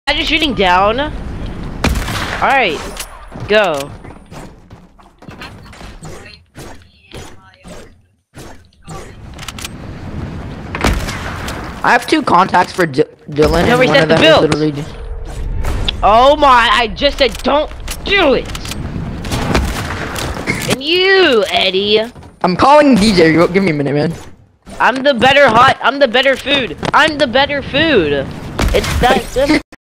Trickshot Video #2
This one is a little bit long, and there are my friends voices because we were in game chat. Just wait til the end. This is not clickbait.